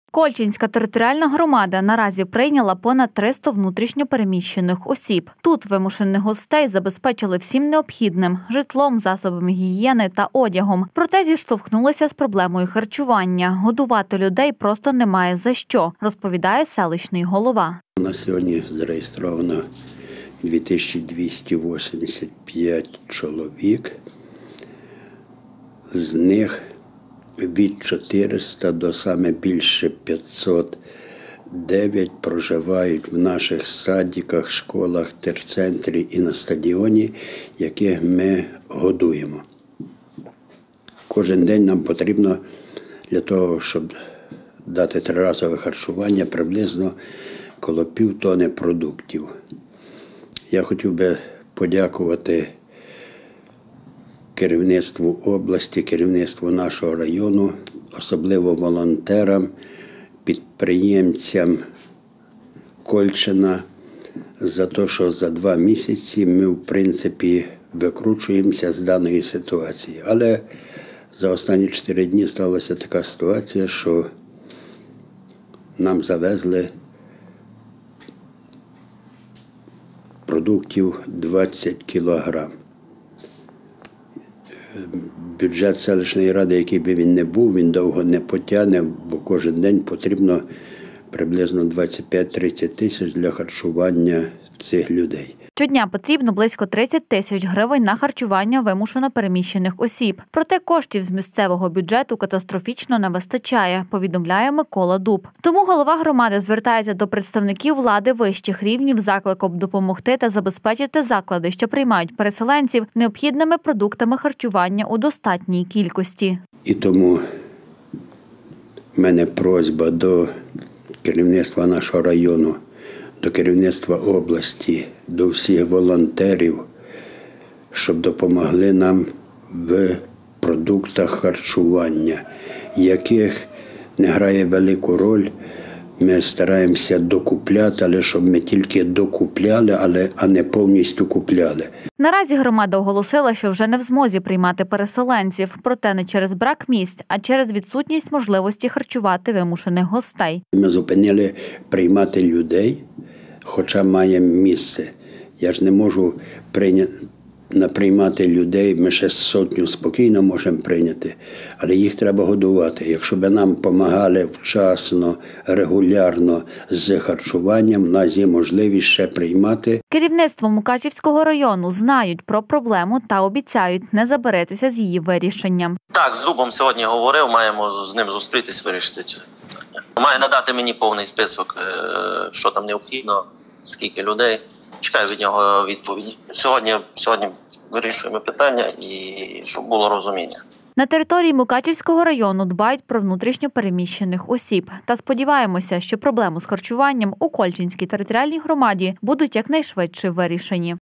Проблеми з харчуванням. Відсутність фінансування
В смт Кольчино наразі проживають близько 300 вимушених переселенців. Переміщених осіб розмістили у закладах освіти по всій громаді. Проте наразі тут зіштовхнулися з проблемою харчування. Годувати людей в такій кількості просто немає коштів. Кольчинська територіальна громада наразі прийняла понад 300 внутрішньо переміщених осіб. Тут вимушених гостей тут забезпечили всім необхідним – житлом, засобами гігієни та одягом. Проте зіштовхнулися з проблемою харчування. Годувати людей просто немає за що, розповідає селищний голова. Щодня потрібно близько 30 тисяч гривень на харчування вимушено переміщених осіб, проте коштів з місцевого бюджету катастрофічно не вистачає – повідомляє Микола Дуб. Тому голова громади звертається до представників вищих рівнів з закликом допомогти та забезпечити заклади, що приймають переселенців забезпечити необхідними продуктами харчування у достатній кількості. Наразі громада оголосила, що вже не в змозі приймати переселенців, проте не через брак місць а через відсутність можливості харчувати вимушених гостей. Керівництво Мукачівського району знають про проблему та обіцяють не забаритися з  її вирішенням. На території Мукачівського району дбають про внутрішньо переміщених осіб та сподіваємося, що проблеми з харчуванням у Кольчинській територіальній громаді будуть якнайшвидше вирішені.